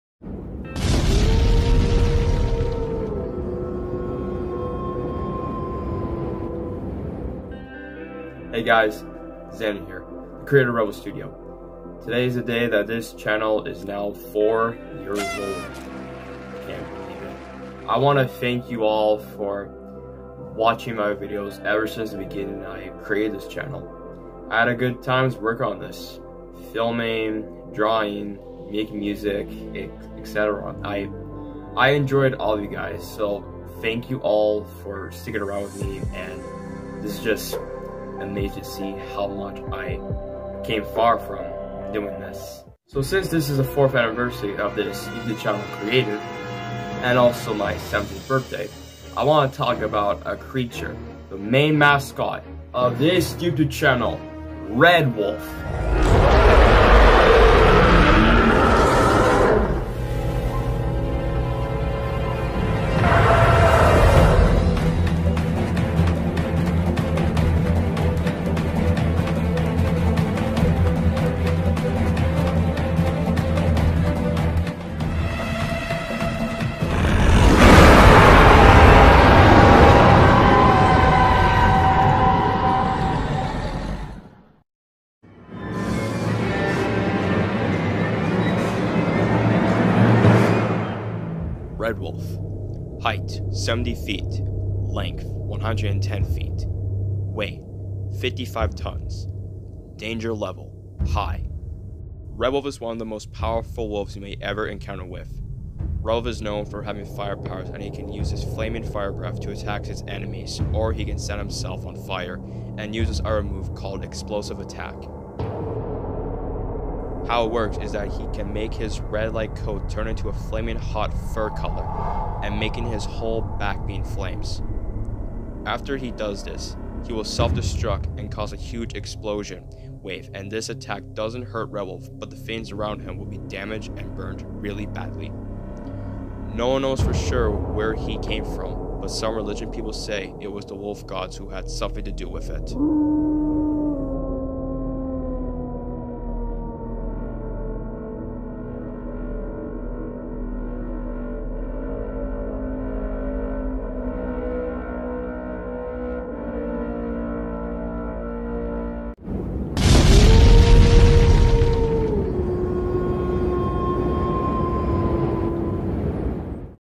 Zander's Mutant Creatures - RedWolf // S1: Episode 1 (4th Anniversary Special)
4th anniversary of me creating this YouTube channel and for this celebration. I did the first episode of Zander's Mutant Creatures and talked about the main Mascot of this channel RedWolf.

And today is also my 17th birthday. Thank you guys for wishing me a Happy Birthday and I had a great day today!